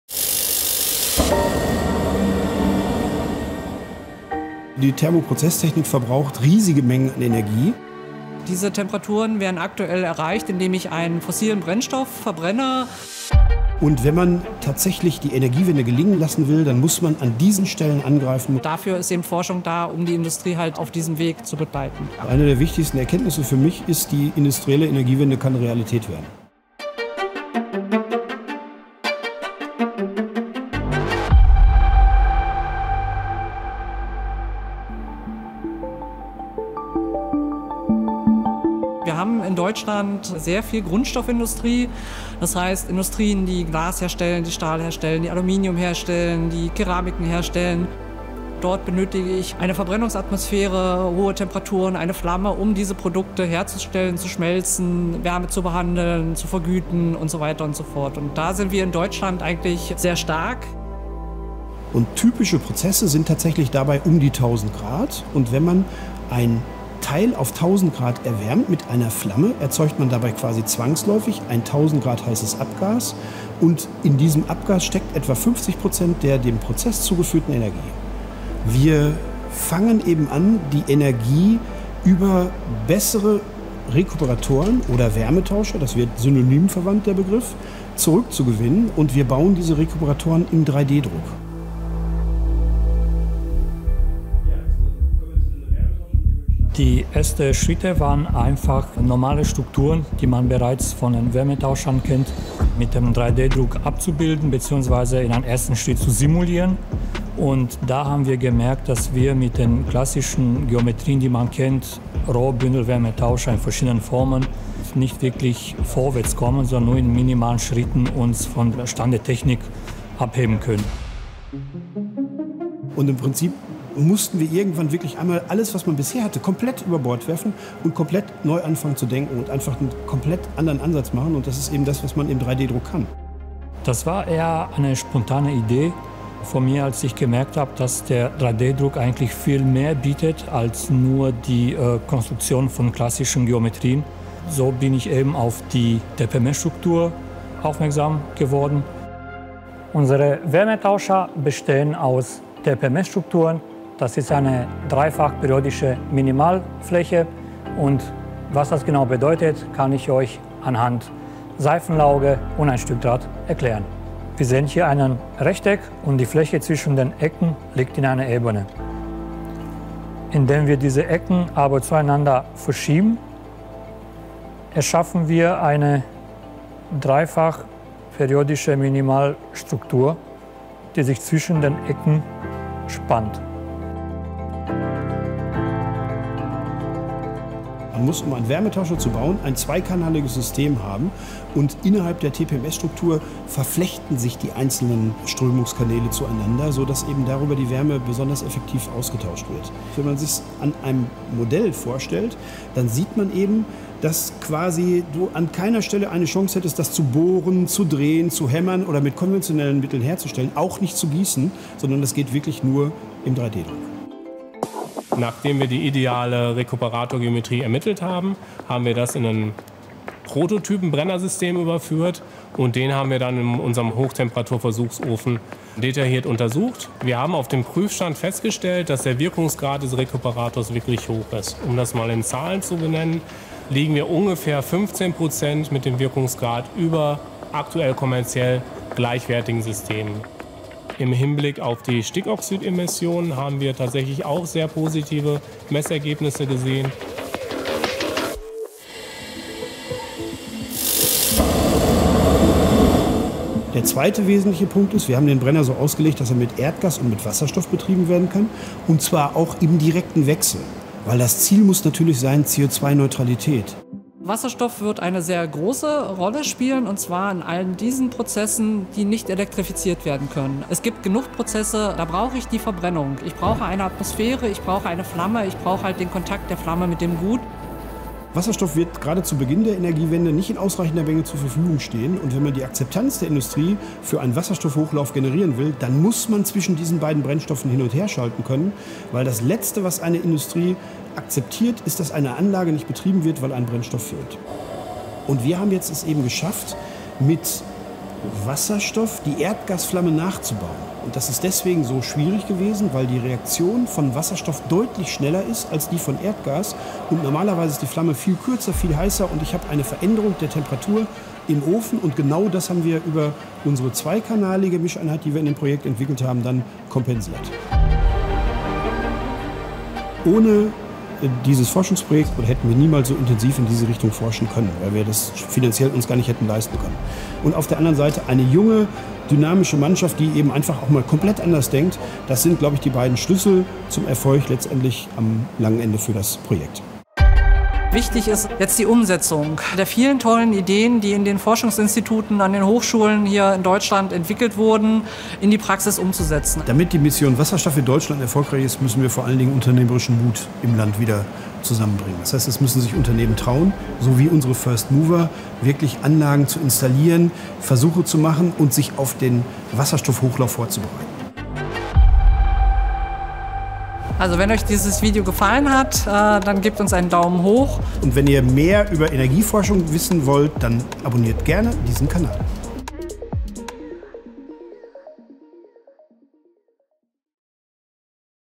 Spart Erdgas und kann Wasserstoff: Dieser Brenner unterstützt die industrielle Energiewende
Viele Produktionsprozesse in der Industrie laufen bei sehr hohen Temperaturen ab oder sind auf eine direkte Flamme angewiesen – zum Beispiel um etwas zu schmelzen, wärmezubehandeln oder zu vergüten. In diesem Fall ist der Einsatz von Strom oder von Wärmetechnologien wie etwa Hochtemperaturwärmepumpen nicht möglich. Im Forschungsprojekt AdReku haben Forschende einen Brenner entwickelt, der deutlich effizienter als herkömmliche Modelle ist und mit bis zu 100 Prozent regenerativem Wasserstoff betrieben werden kann. Das Besondere daran: Über einen im 3D-Druck gefertigten Rekuperator (Wärmetauscher) wird möglichst viel Wärme aus der Abluft wiedergewonnen und in den Verbrennungsprozess zurückgeführt. Im Video erklären die Forschenden Jens te Kaat und Dan-Adrian Moldovan von Kueppers Solutions sowie Dr. Anne Giese und Marcel Biebl vom Gas- und Wärme-Institut GWI Essen, wie sie auf die Idee für den Brenner gekommen sind und warum es wichtig ist, Sachen einfach mal ganz neu zu denken.

Das Forschungsprojekt AdReku wurde vom @bundeswirtschaftsministerium (BMWK) mit fast einer Millionen Euro gefördert.

Teil des Projekts sind @kuepperssolutions und das @GWIEsseneV.

Mehr zu Fördermöglichkeiten und Fakten rund um die angewandte Energieforschung des BMWK

Mehr Informationen rund um die Mission Wasserstoff im 8. Energieforschungsprogramm des BMWK

Produktion: Ein Film von filmreif.biz im Auftrag des Projektträgers Jülich, Forschungszentrum Jülich GmbH, 2024. Der Dreh fand am 24. Juli 2024 beim Gas- und Wärmeinstitut Essen statt. industrie energiewende brenner thermoprozesstechnik 3ddruck additive-fertigung